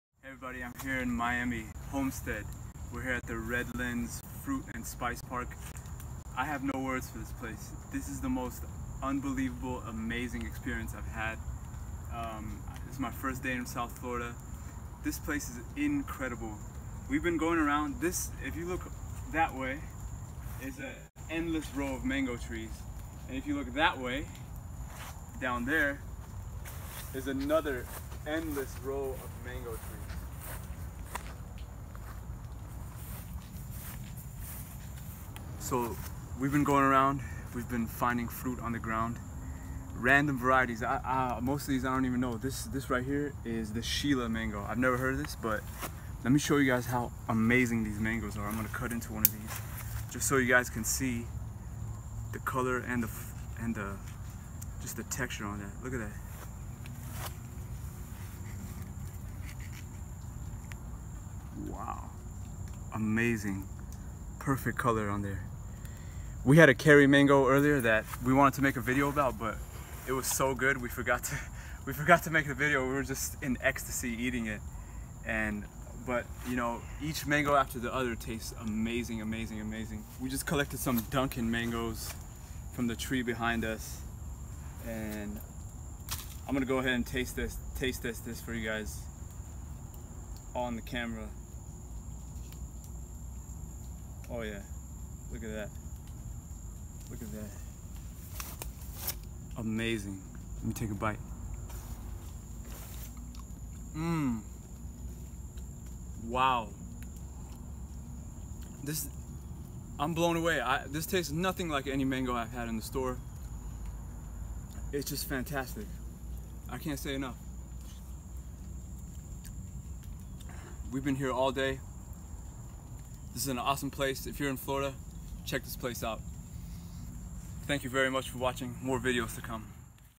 Eating Mangos At The Fruit and Spice Park
We're visiting the Redlands Fruit and Spice Park in south Florida. Incredible place that is open to the public!